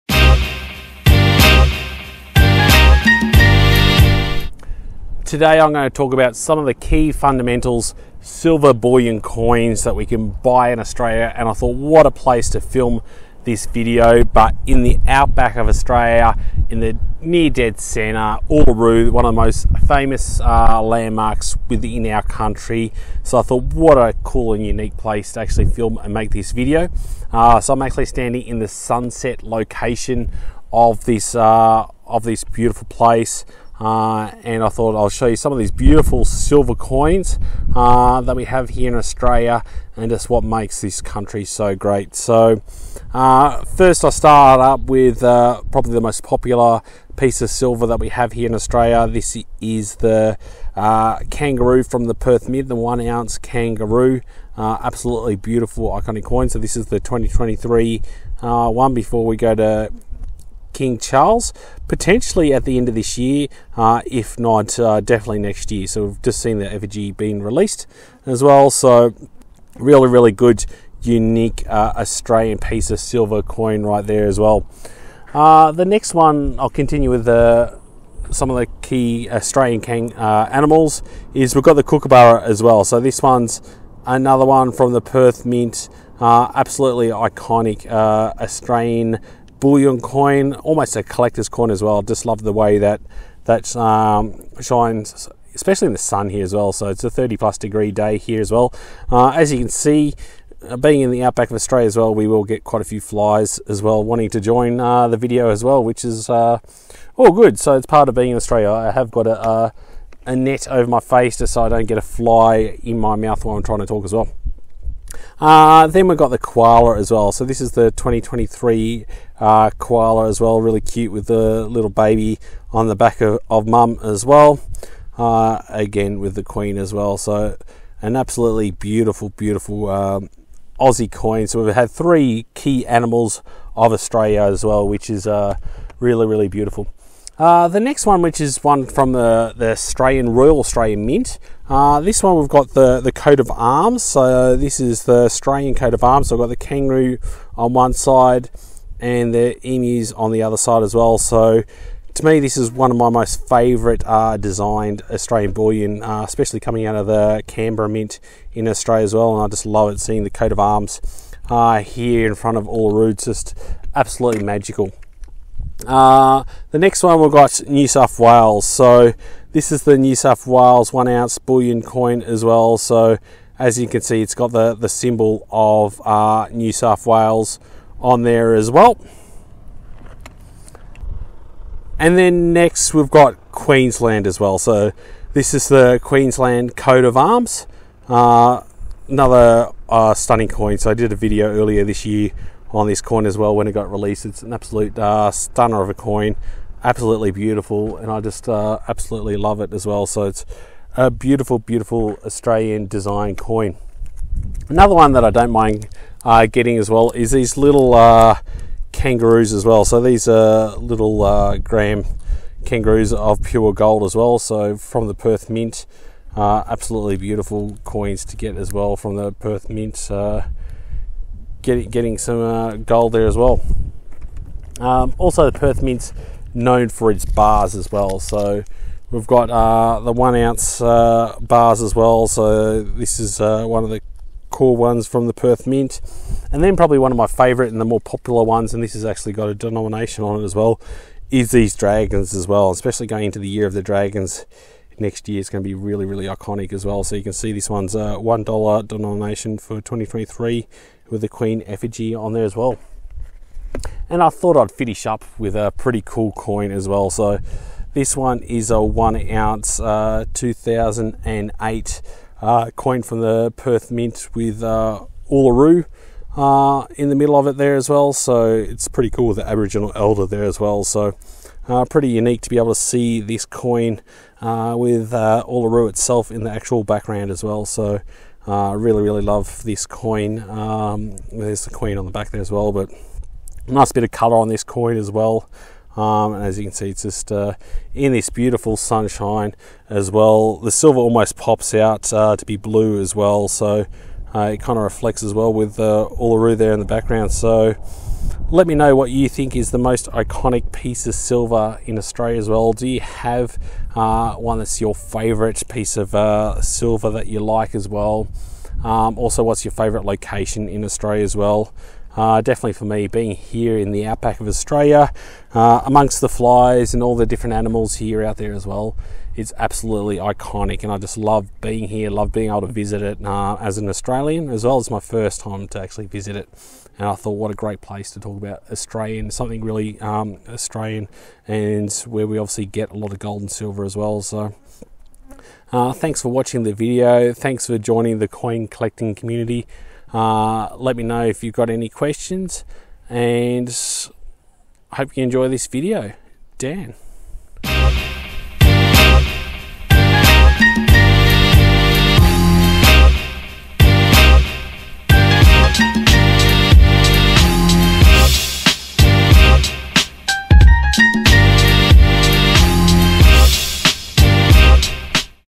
BEST Australian Silver Coins – Filmed in the Outback at Uluru
Reviewing the best of Australian Silver in the Outback at Uluru, what a perfect Australian combination

See you on my next video! Dan

Intro: (0:00)
Kangaroo: (0:31)
Kookaburra: (1:14)
Koala: (1:50)
AUS Coat of Arms: (2:16)
NSW Coat of Arms: (2:45)
QLD Coat of Arms: (3:00)
$2 Gold Kangaroo: (3:24)
Kangaroo Bar: (3:44)
Dragon Bar: (3:56)
Discover Australia Uluru: (4:13)
Outro: (5:35)